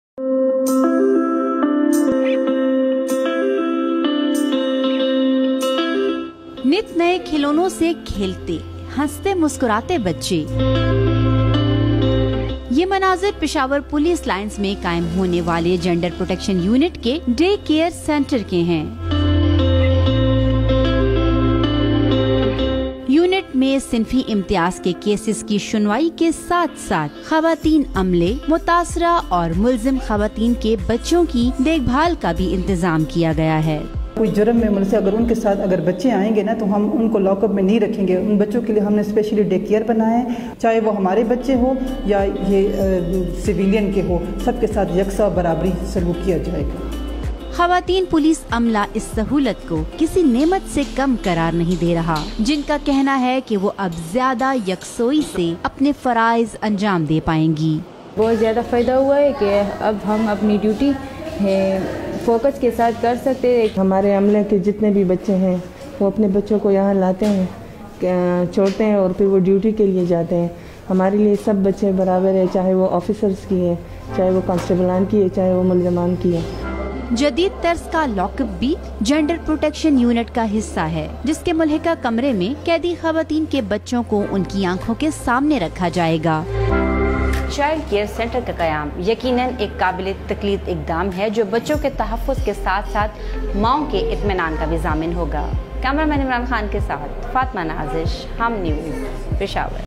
Establishment of GPU in Peshawar || Group Development Pakistan
Hum News report on establishment of GPU in Peshawar